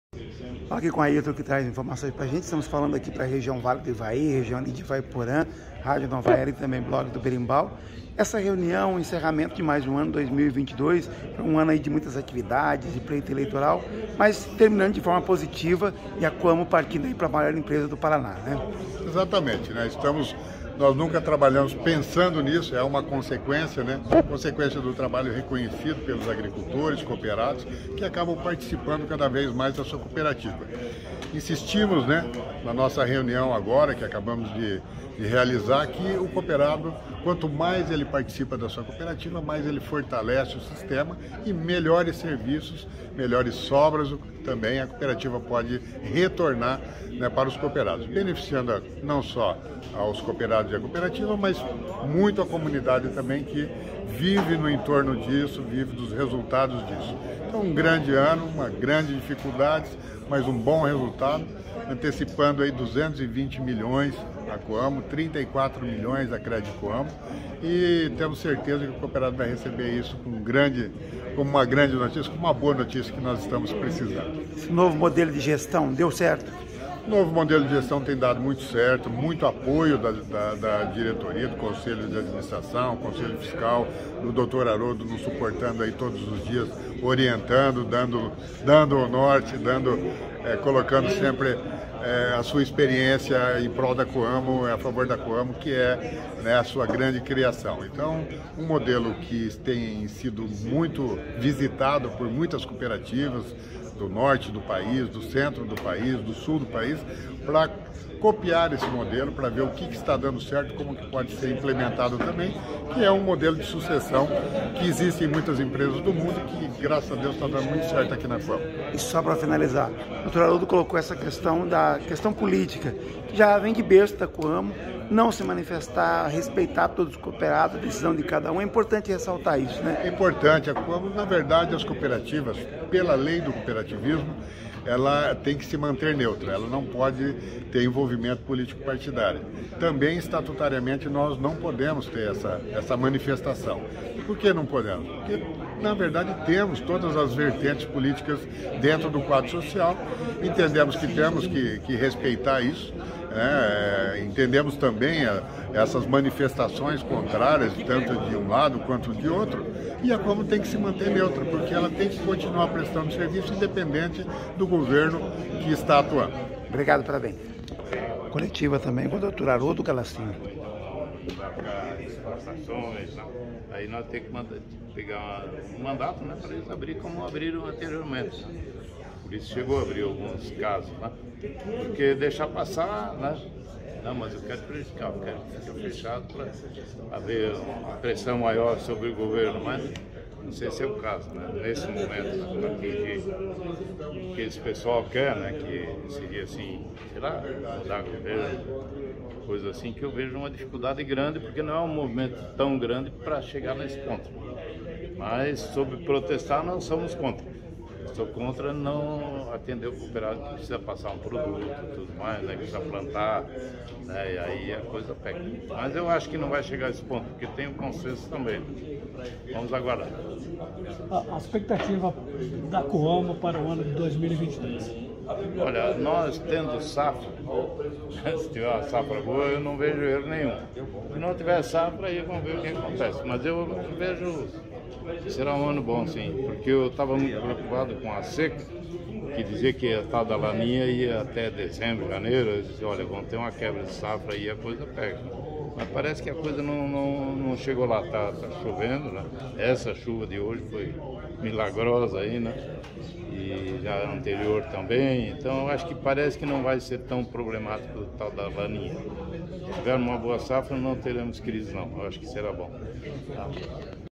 COAMO - Cooperativa anuncia antecipação de sobras de R$220 milhões
A antecipação de parte das sobras de cada Exercício já é uma tradição na Coamo Agroindustrial Cooperativa. Na manhã desta terça-feira, 22, a diretoria da cooperativa anunciou aos cooperados durante Reunião de Campo virtual, o valor de R$ 220 milhões que será distribuído no dia 05 de dezembro para os mais de 30 mil cooperados, conforme a movimentação de cada um na cooperativa na entrega da soja, milho e trigo e aquisição de insumos. Esse é um momento aguardado com expectativa pelos agricultores associados e as comunidades na área de atuação da cooperativa no Paraná, Santa Catarina e Mato Grosso do Sul. Na oportunidade, a Credicoamo Crédito Rural Cooperativa também anunciou o pagamento dos juros sobre o capital aos associados, no valor superior a R$ 34 milhões. O valor será pago no dia 12 de dezembro, em todas as agências da cooperativa de crédito. Os cooperados que têm os Aplicativos Coamo e Credicoamo poderão acessar o valor depositado diretamente nos aplicativos. O presidente dos Conselhos de Administração da Coamo e Credicoamo, José Aroldo Gallassini, destaca que a tradição de antecipar as sobras é um benefício comemorado pelo quadro social. “Os cooperados sabem que dezembro é o mês de antecipação das sobras, é um momento aguardado, e isso só é possível devido a solidez das cooperativas [Coamo e Credicoamo]”, frisa. A respeito dos juros sobre o capital, uma novidade implantada no ano passado, beneficia a todos de duas formas: primeiro pelo retorno do valor ao associado e, segundo, por meio da capitalização da Credicoamo, que terá mais recursos para financiar os próprios associados. “Parte desse juro, o associado pode utilizar como bem entender e a outra parte será capitalizada na conta capital, aumentando assim, o capital de giro da cooperativa para que possa disponibilizar ainda mais recursos para os financiamentos do quadro social”, diz José Aroldo Gallassini. Além dos juros sobre o capital social, o associado continuará recebendo as sobras da Credicoamo, após a Assembleia Geral Ordinária, que será realizada em março de 2023. “De tudo que a Credicoamo faz, tem uma margem que fica para a capitalização e para as sobras. Quanto às sobras de 2022, serão semelhantes à de anos anteriores, ou seja, significativa e na proporção da movimentação financeira. Isso é importante e somente o sistema cooperativista faz isso: devolver o resultado aos seus sócios.”